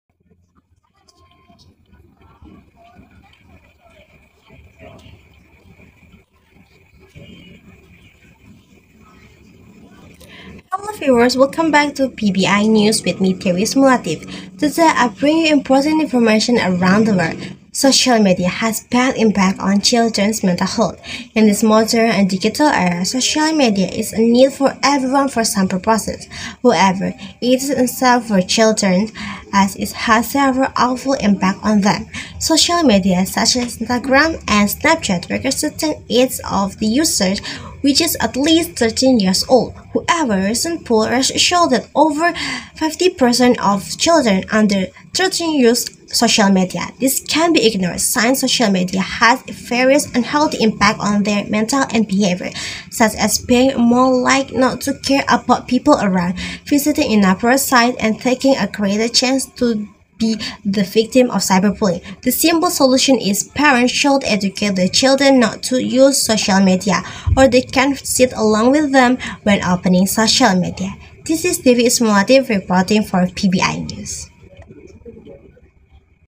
Tik Tok News Anchor Competition 2022_DEWI ISMUL LATIF_SMAN 1 DEMAK
Program Studi Pendidikan Bahasa Inggris Universitas PGRI Semarang proudly present "Tik Tok News Anchor Competition 2022"

Kegiatan Tik Tok News Anchor Competition 2022 merupakan kegiatan tahunan yang di selenggarakan oleh Program Studi Pendidikan Bahasa Inggris Universitas PGRI Semarang untuk memberi wadah kepada siswa-siswi SMA Sederajat mengembangkan potensi dalam kecakapan membawakan berita dan melatih public speaking.

Mau tau pemenang Tik Tok News Anchor Competition 2022?

follow instagram kami @pbiupgris

Regards,
Committee of Tik Tok News Anchor Competition 2022